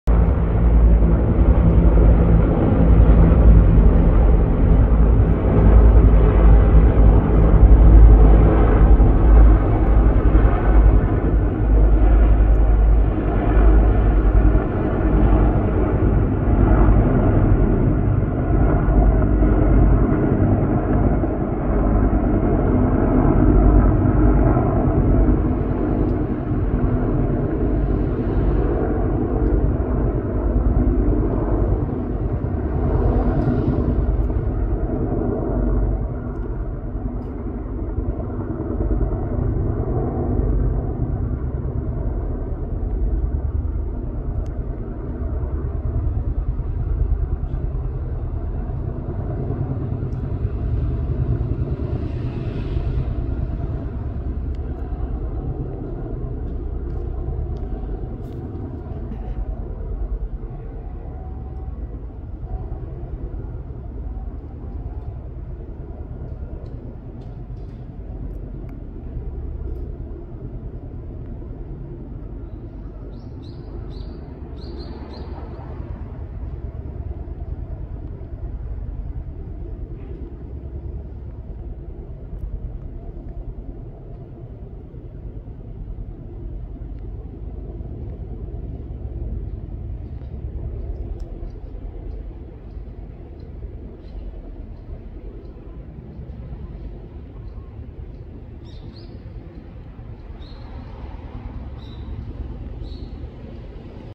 なんかの訓練？ 自衛隊のヘリコプターが5機も飛んでた（京都府宇治市）（2023/11/16）
自衛隊のヘリコプターが、13時頃滋賀方面から大阪方面へ上空を飛んで行きました。
何かの訓練でしょうか？